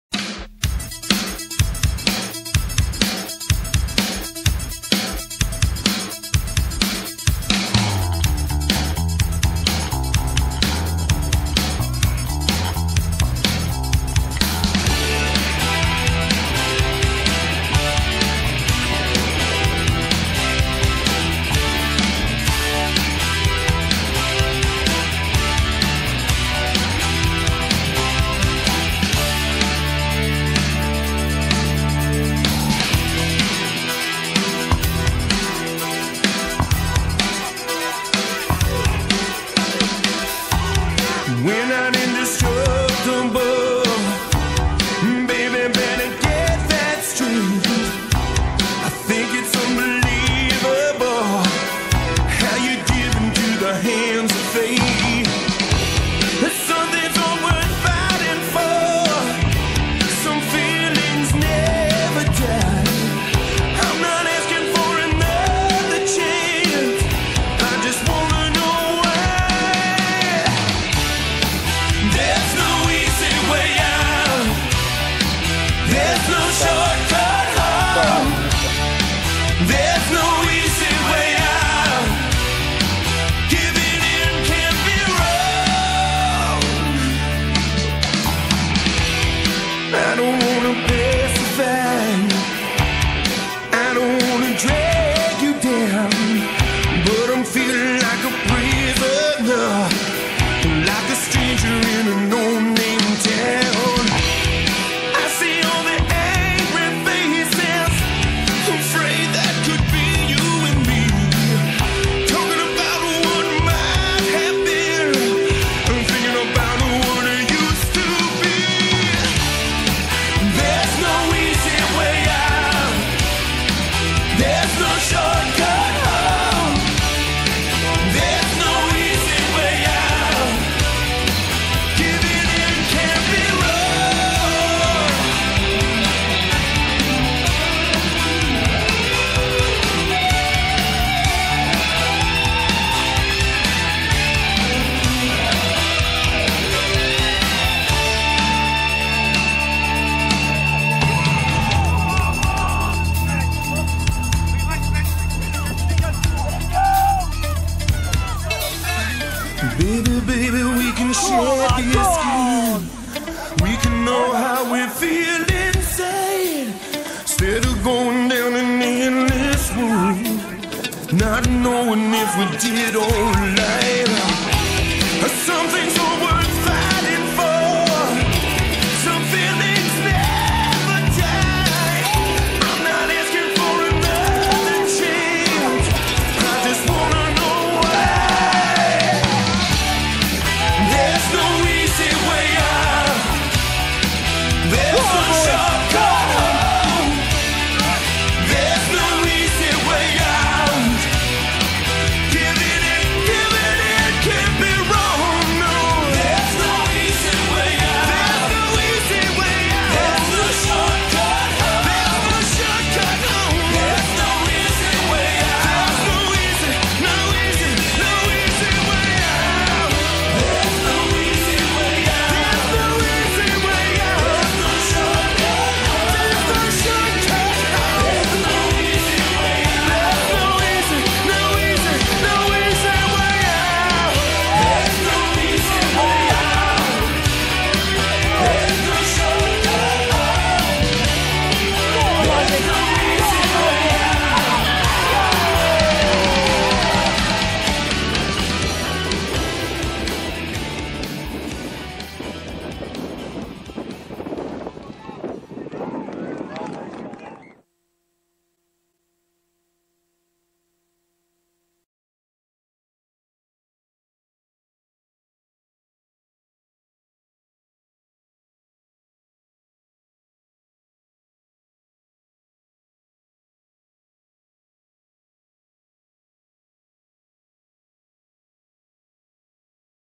Norland Sharks 4 Sharlston Rovers 12 - Yorkshire Cup 09 Semi Final
Rovers make their way through to a third consecutive Yorkshire cup final with tough victory over Norland Sharks in Hull.